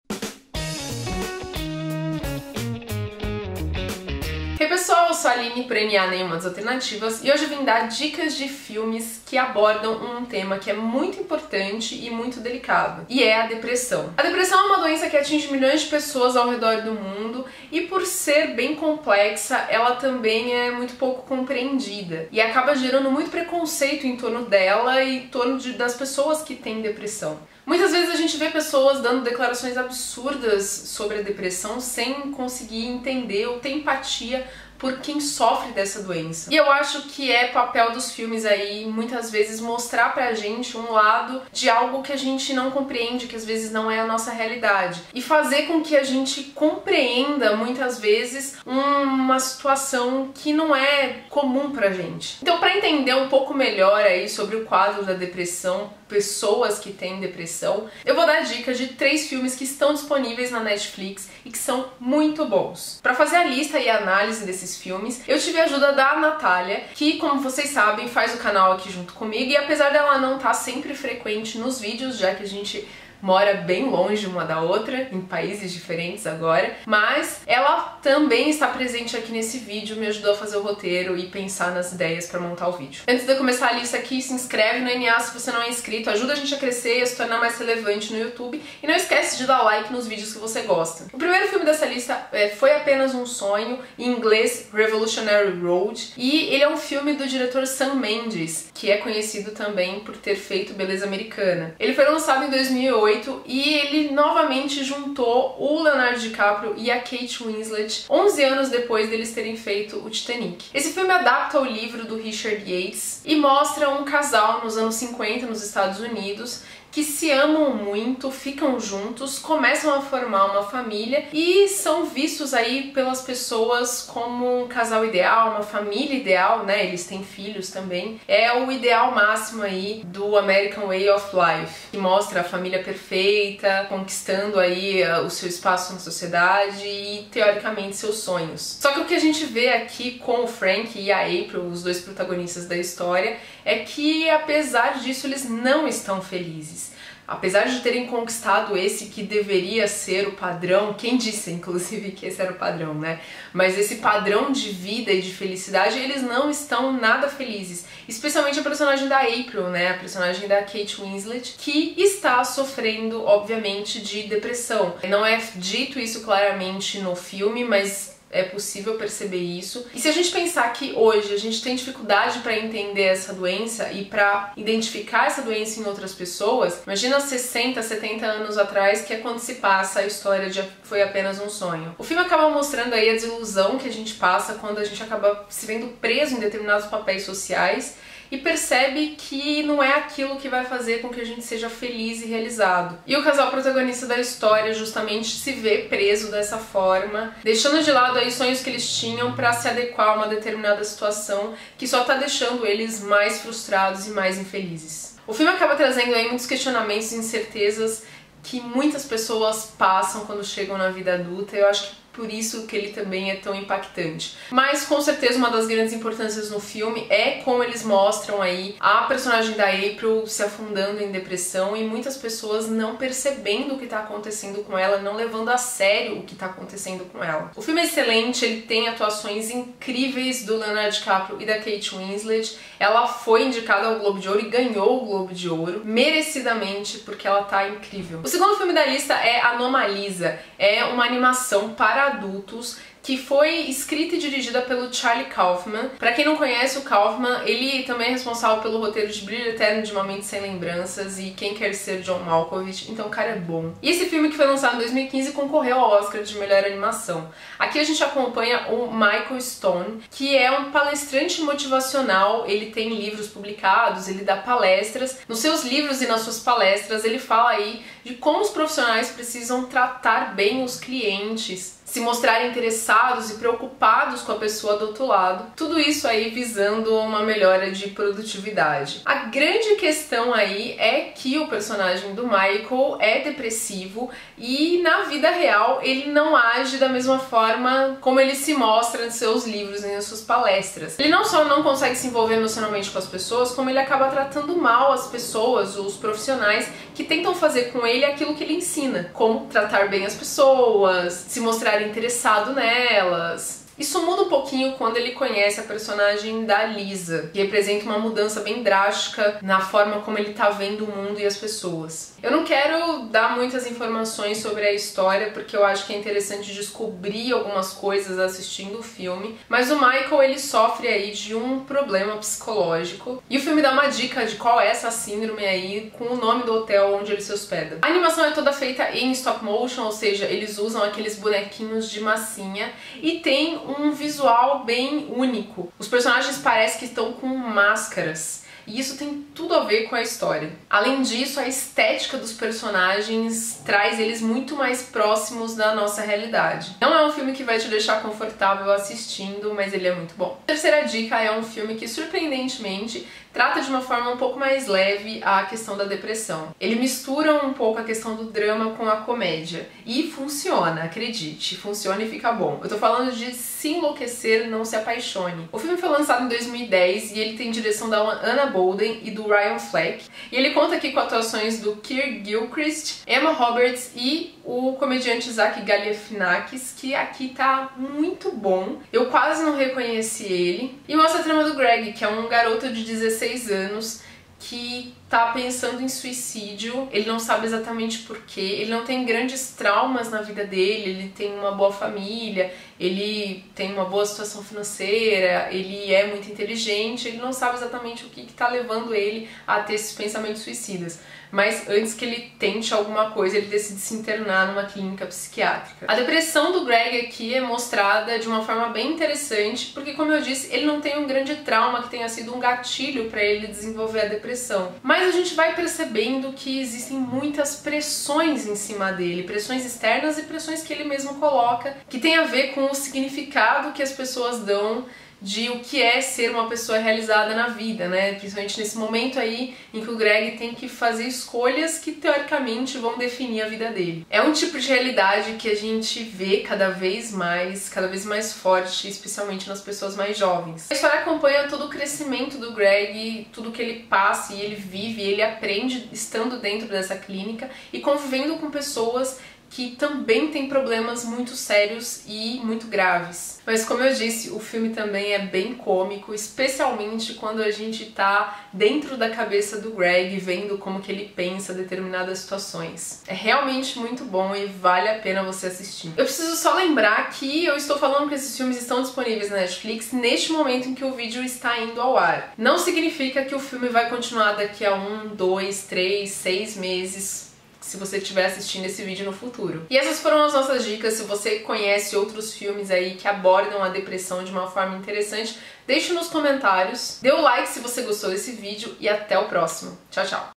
3 FILMES NA NETFLIX PARA ENTENDER A DEPRESSÃO
A depressão é uma doença séria e os filmes nos ajudam a entender melhor ela e quem passa por quadros depressivos. Vem ver a lista com filmes na Netflix que ajudam a entender melhor a depressão.